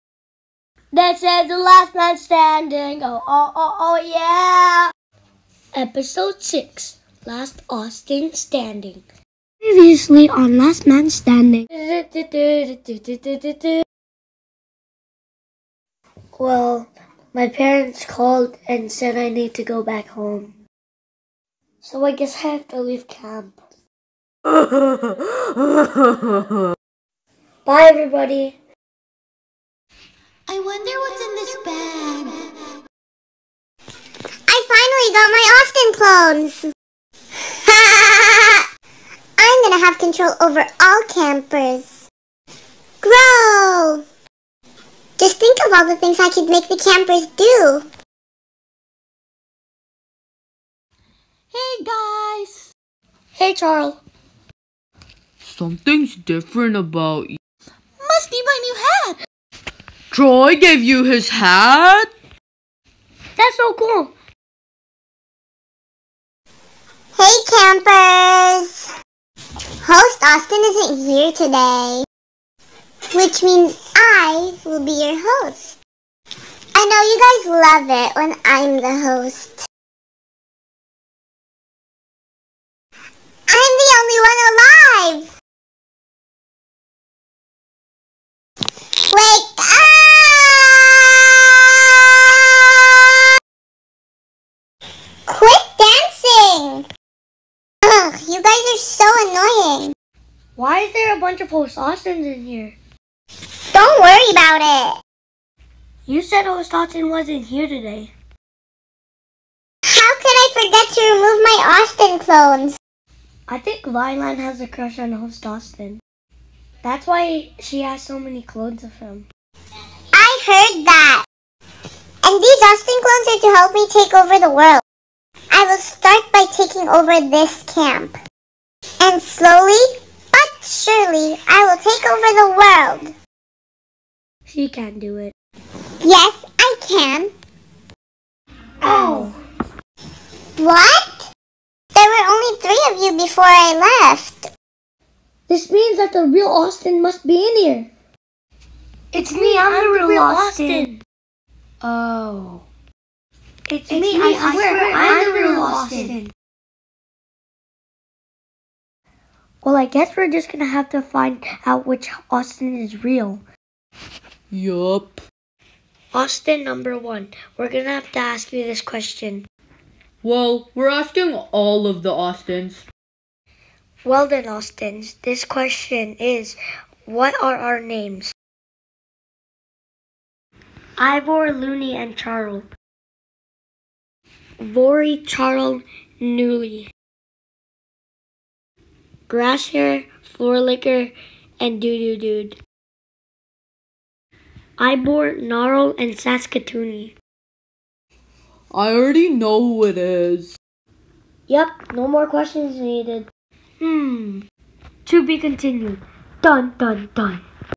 Last Man Standing (season 1, Episode 6: Last Austin Standing) || voiced Gacha series
An elimination show on gacha! Inspired by Total Drama Island.

Charle, Luni and Ivor find a bunch of Austin clones but which one is the real Austin?!